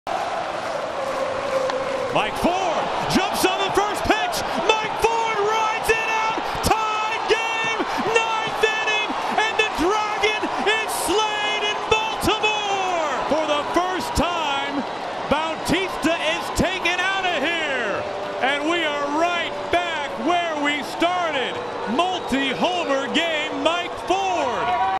MIKE FORD TIES THE GAME WITH A SOLO HOMER OFF FELIX BAUTISTA!!!
MIKE FORD IS THE GOAT

and we still lost...